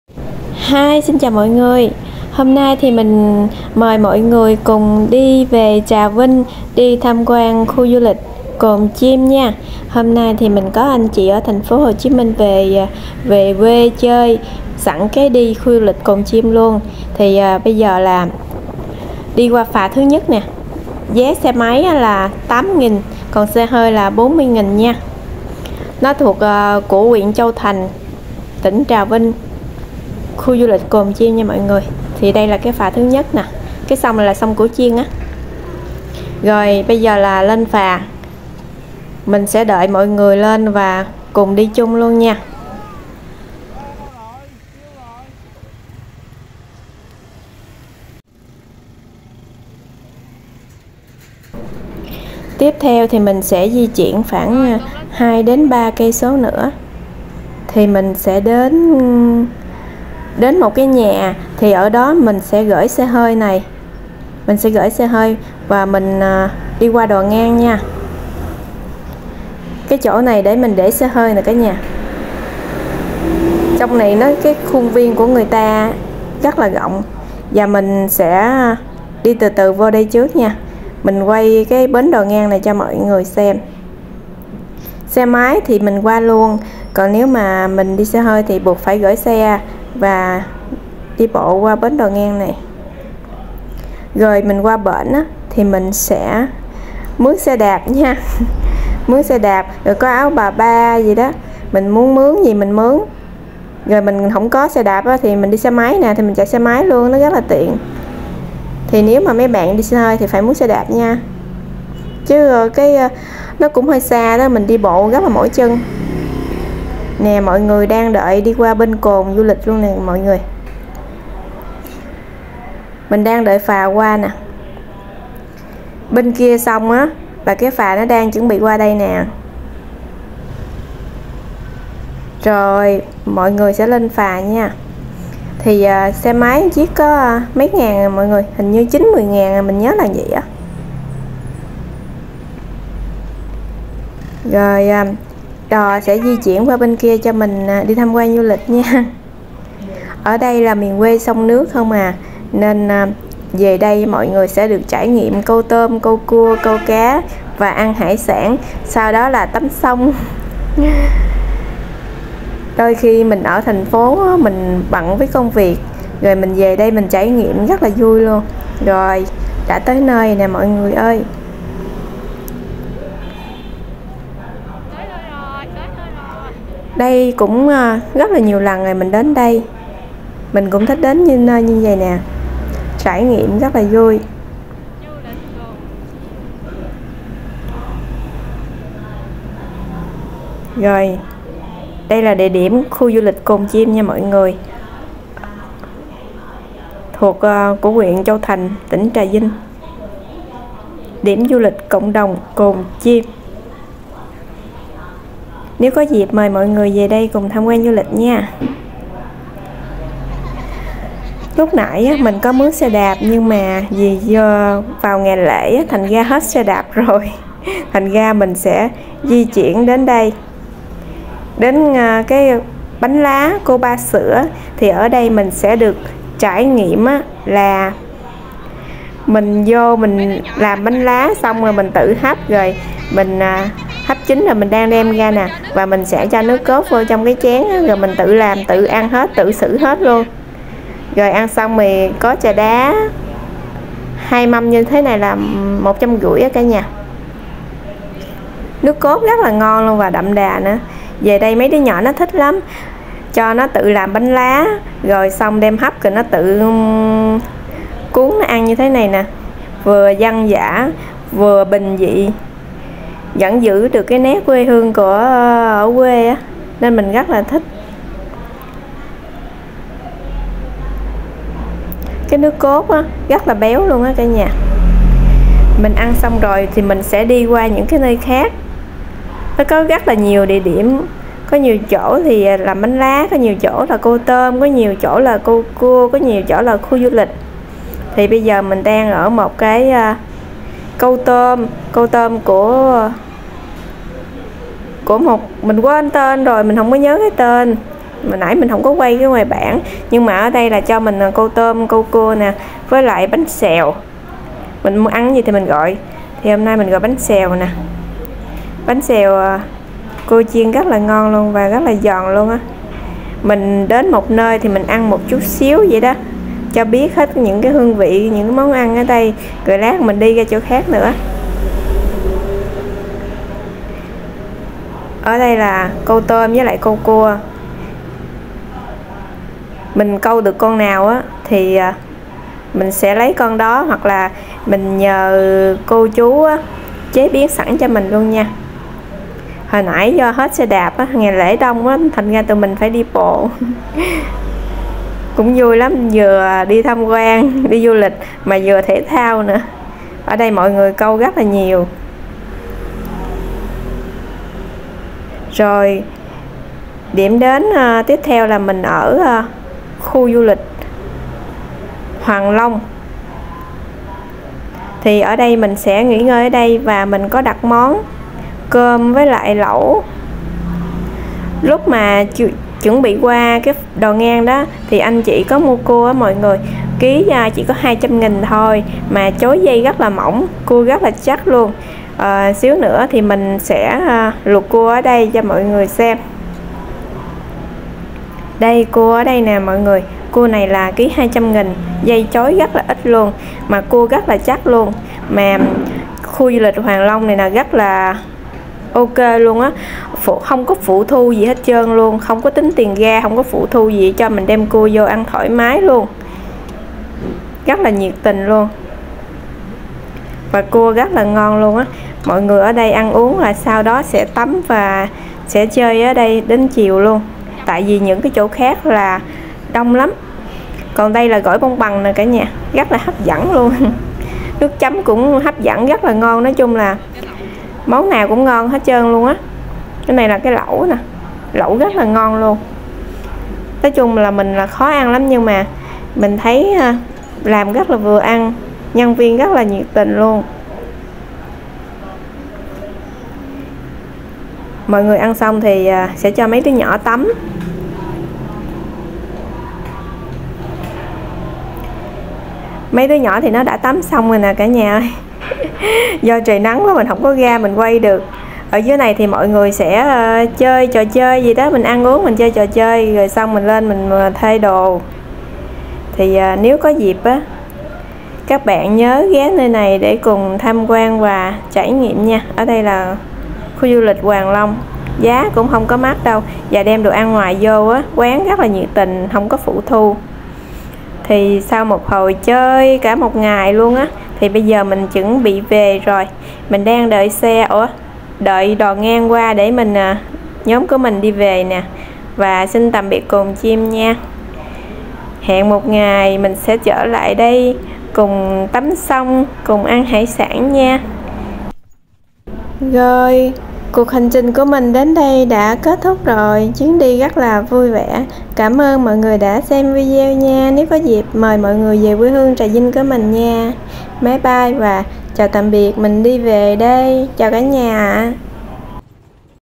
Khu Du Lịch Cộng Đồng Cồn Chim, Trà Vinh,Người Quê Chỉ Có Tấm Lòng
Khu du lịch cộng đồng Cồn Chim, Trà Vinh, đây là vùng quê của sông nước, tôm, cua, cá và các món ăn dân gian, bánh xèo, bánh lá, mang đậm nét miền tây quê Huơng, các bạn sẽ đuợc câu tôm, câu cua, tắm sông, ăn hải sản, tận hưởng những phúc giây bình yên giữa cụôc sống xô bồ,...